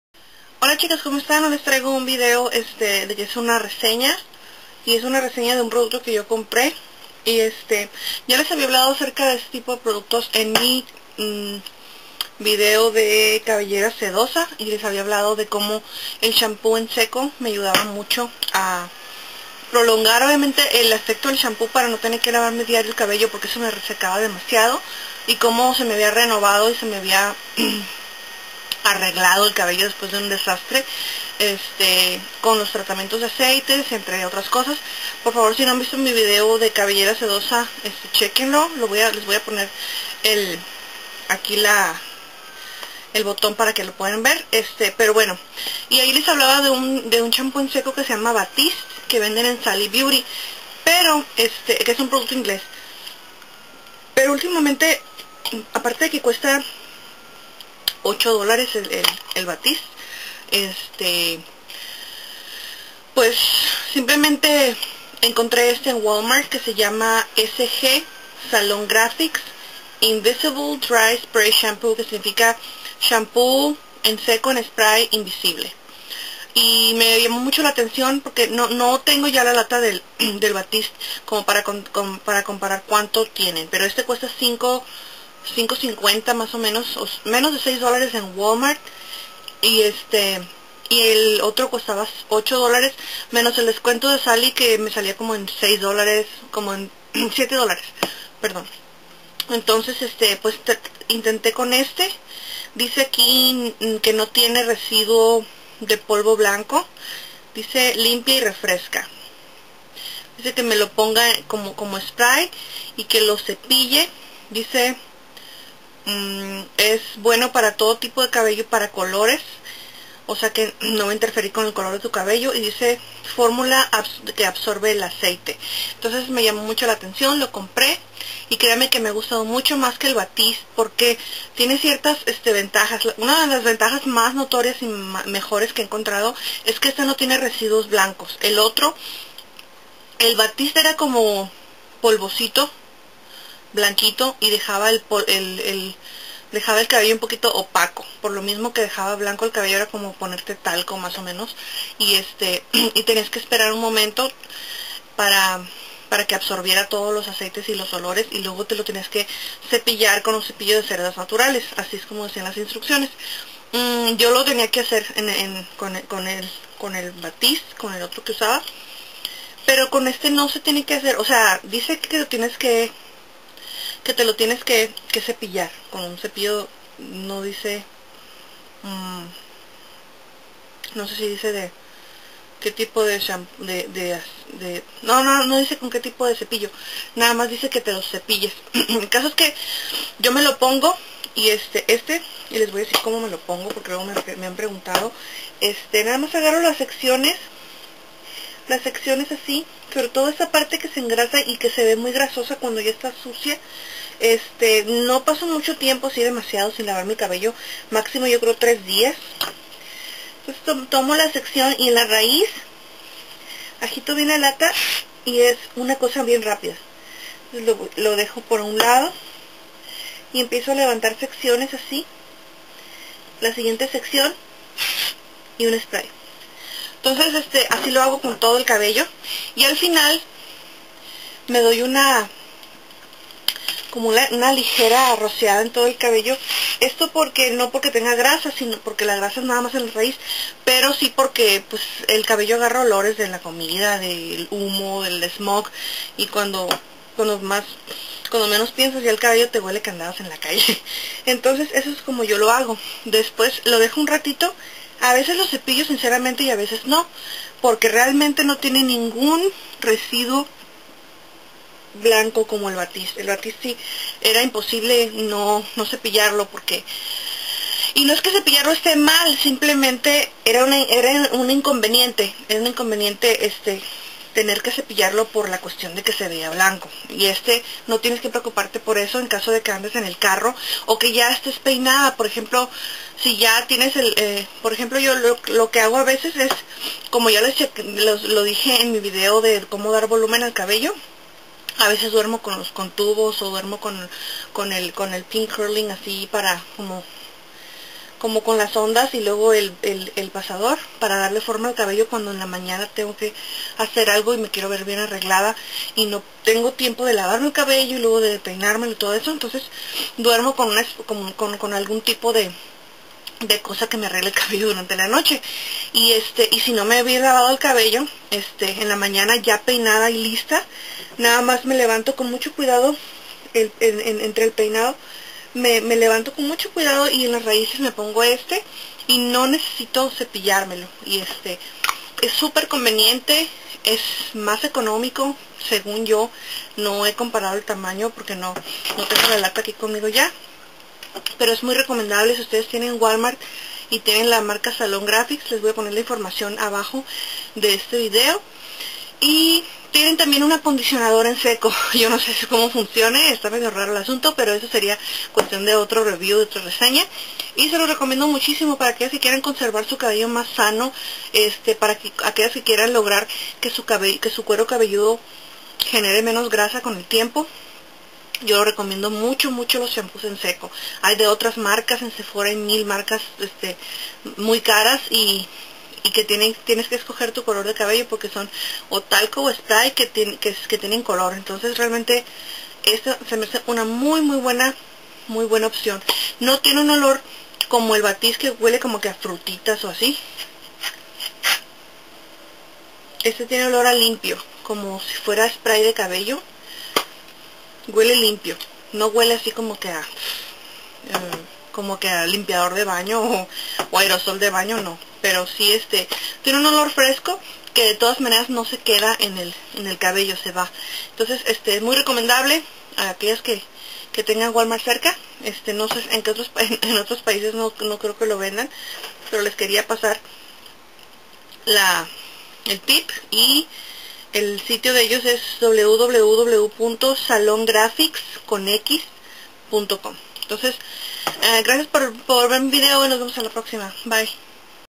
Reseña: Salon Grafix Dry Shampoo comparado con Batiste
Sorry chicas, no se donde mas, en que otras tiendas o paises lo venden... No conozco ningun shampoo en seco ademas de estos dos.

Esta resena es mi punto de vista personal acerca de un producto que compre yo misma.